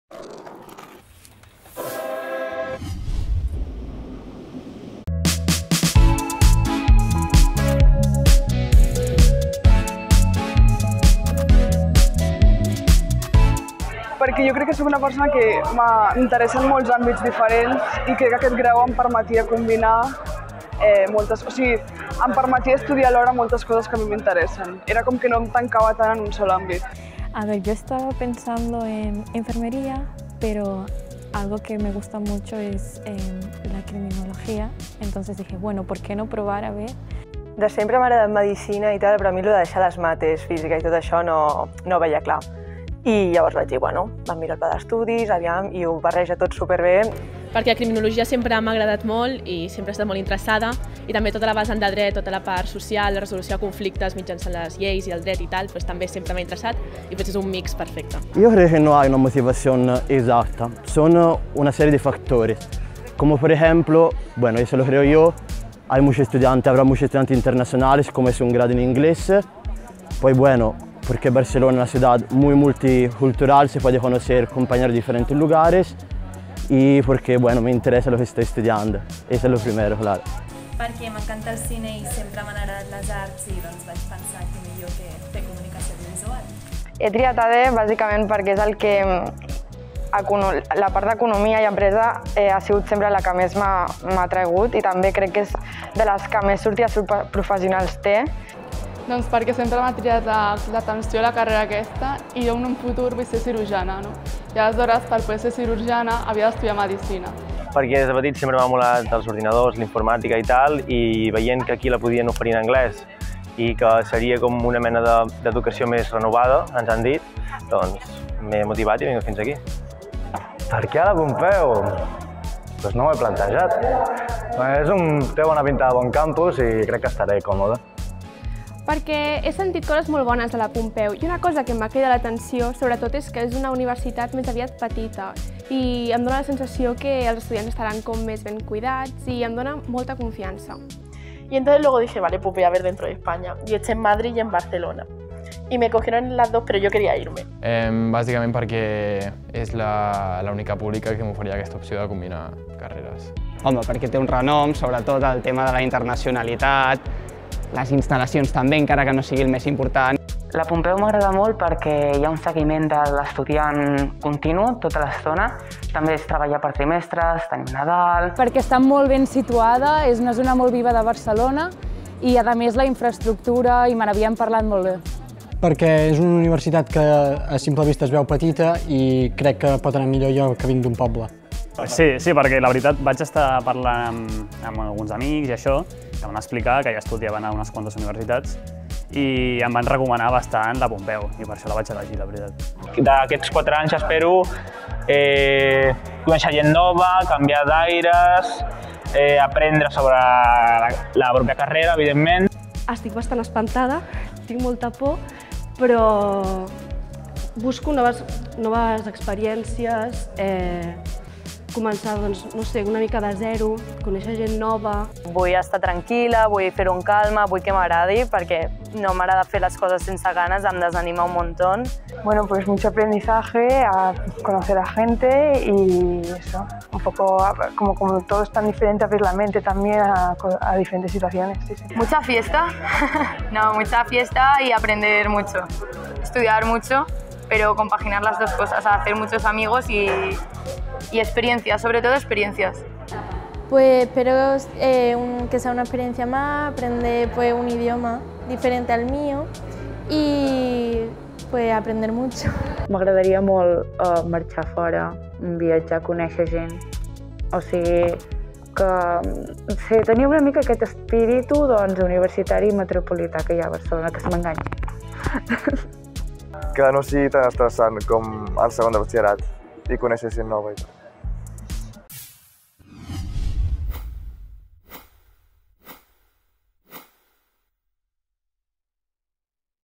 Les noves cares del curs 2021-2022 a la UPF
Qui són els nous estudiants? Quins estudis han triat? Per què han triat la UPF? I el més important, què esperen del seu pas per la Universitat? Benvinguts a la UPF!